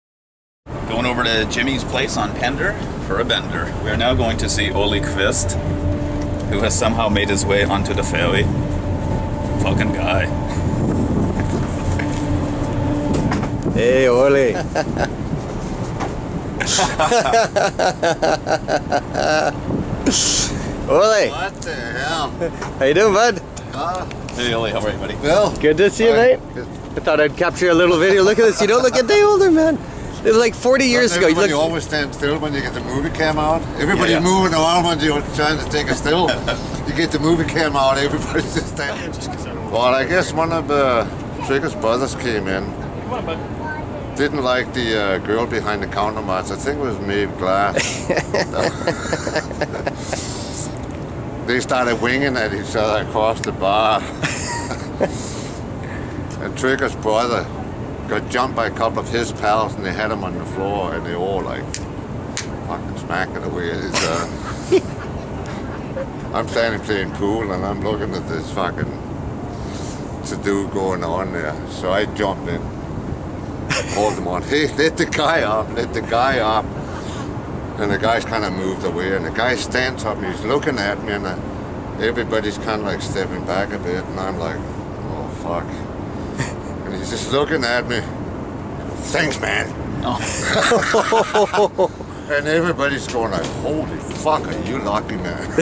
Good story by Ole Qvist
RIP Ole Qvist.

The Westside O-Men bumped into Ole on the ferry to Pender back in 2011.  What a great guy.  Always that smile . . .